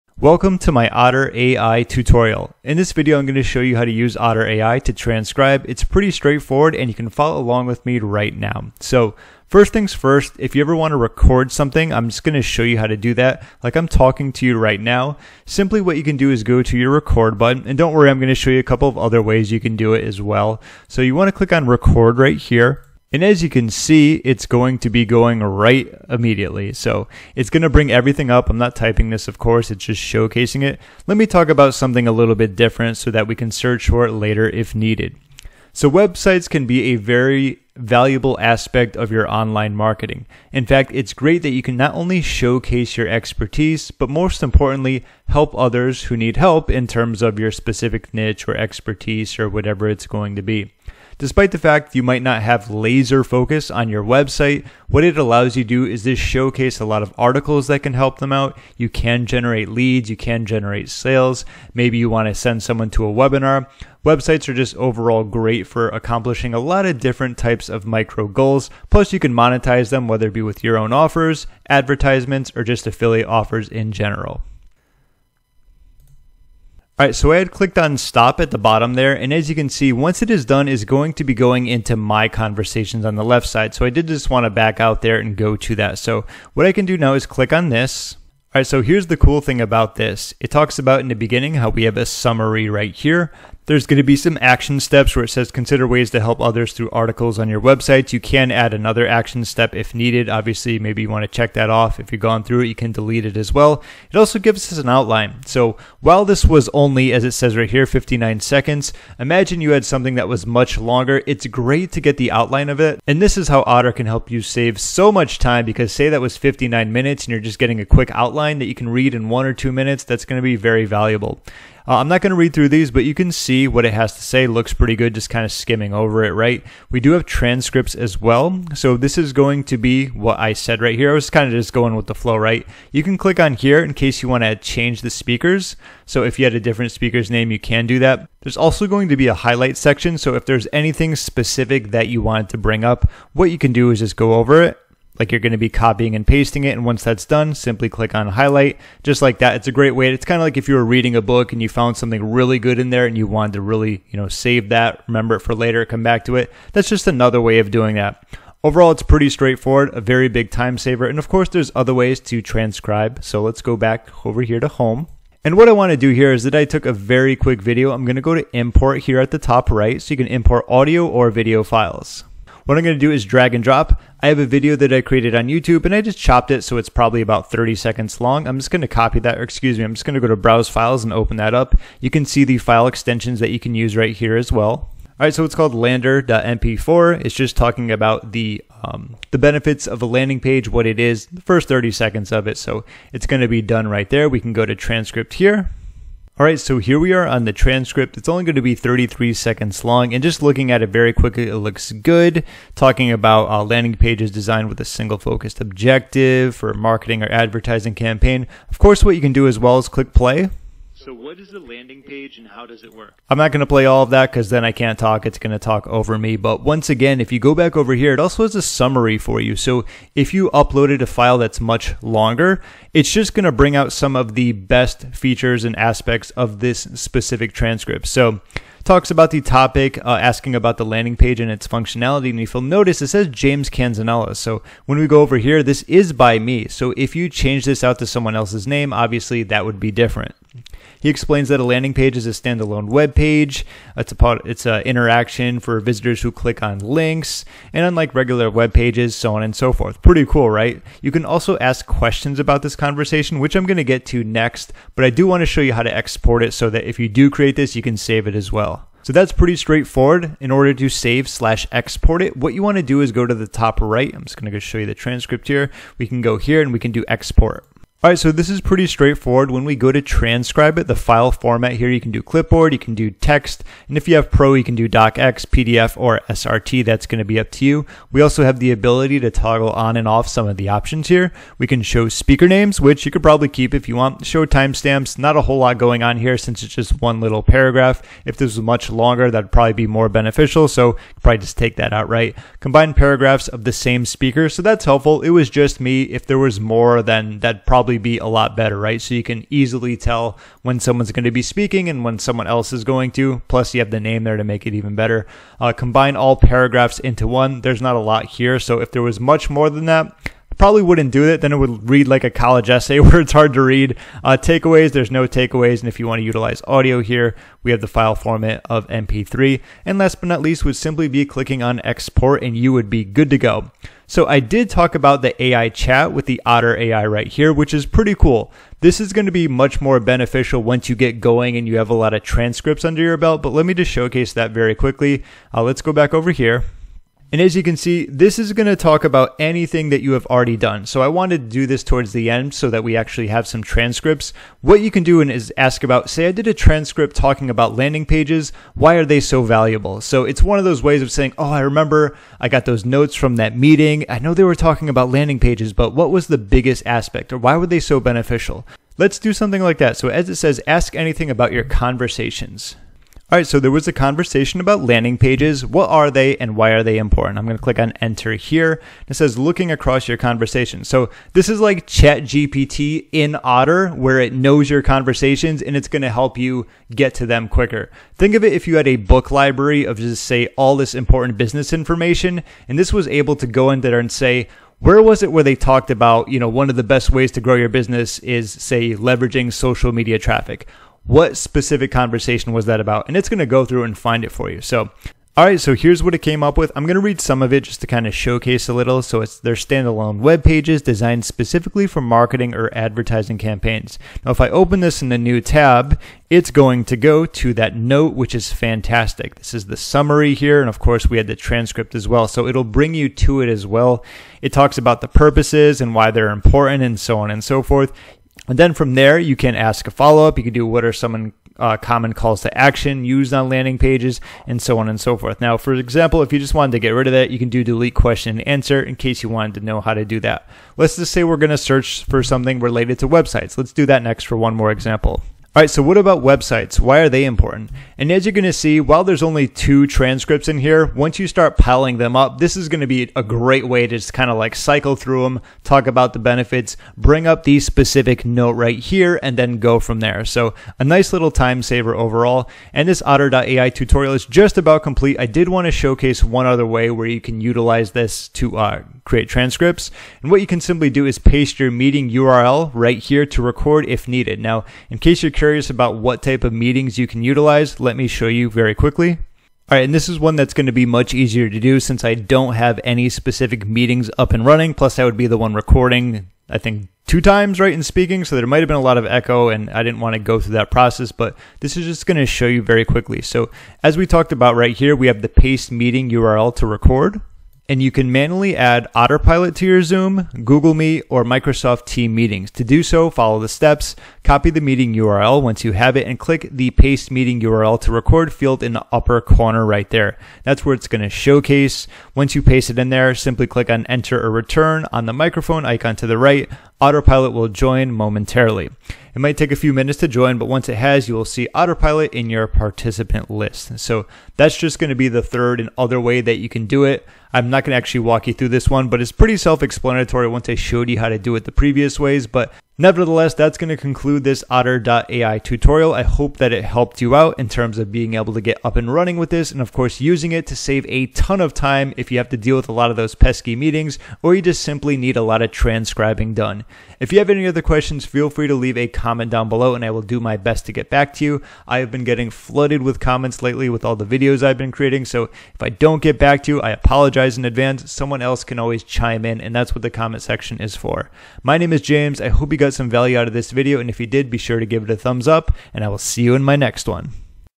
Otter AI Tutorial (How To Use Otter AI To Transcribe)
Otter AI Tutorial For Beginners

⌚ Chapters
00:00 Introduction
00:10 Otter AI Recording
01:28 Otter AI Transcript & Summary
02:45 Otter AI Import
04:14 Otter AI Export
05:37 Otter AI Chat
07:40 Meeting URLs
08:42 Outroduction

👋 Hey there! Thanks for checking out my Otter Tutorial For Beginners video. Here's what I'll be covering for you:

🏝️ Otter Tutorial - We'll dive deep into the Otter AI waters, exploring all its amazing features that make transcribing as easy as a day at the beach.

🏝️ How To Use Otter AI To Transcribe - Learn how to effortlessly turn your speech into text, just like finding hidden treasure in the sand!

🏝️ Otter AI Chat - I'll show you how to chat with Otter AI like it's your new tropical buddy, helping you stay organized and on top of your game.

🔍 Hope you enjoy the video! If it helped you navigate the seas of Otter AI, don’t forget to give that thumbs up a click. It’s like sending a little message in a bottle of encouragement my way!